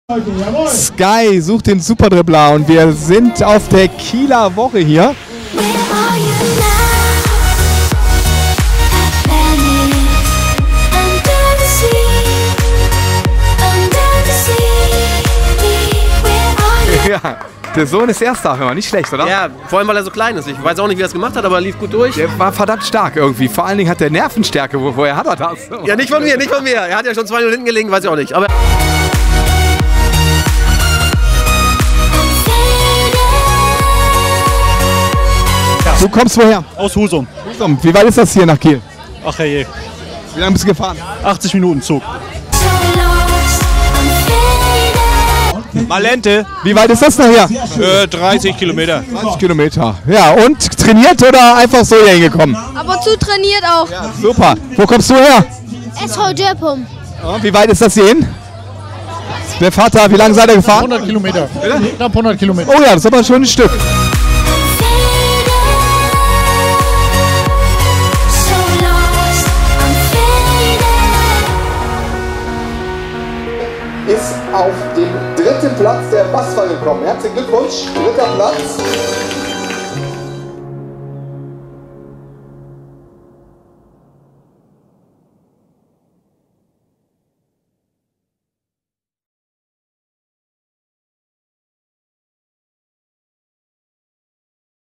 Superdribbler Regionalfinale Nord auf der Kieler Woche
Bilder vom Regionalfinale von sky Supermarkt sucht den Superdribbler von der Kieler Woche 25/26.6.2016